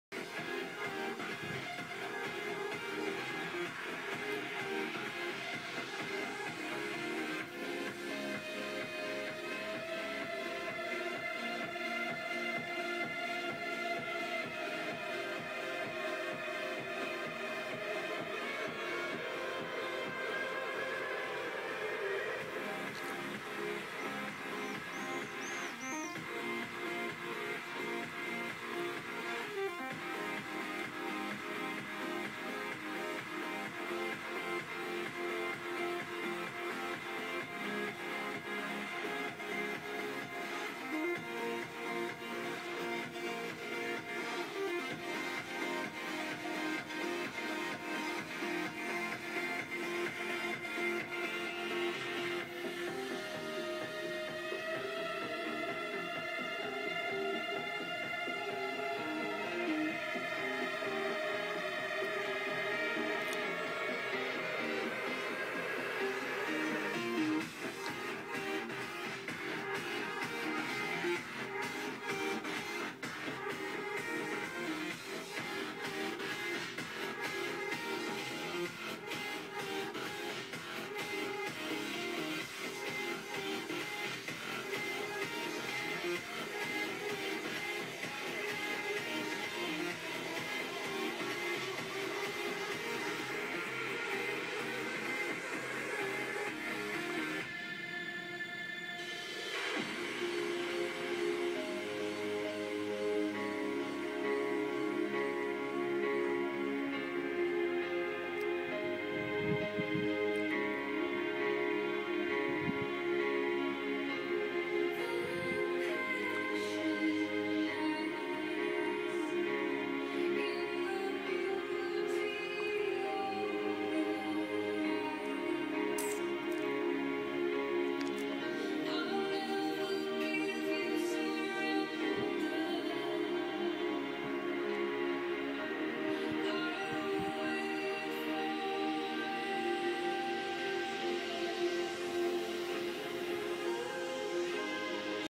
IMG 7326-05.07.2014-black things that appear and dissapear in the sky
they look from the far like circles but then like birds maybe that are souls that can through there energybody take every shapp they see and know or foiund out in time:)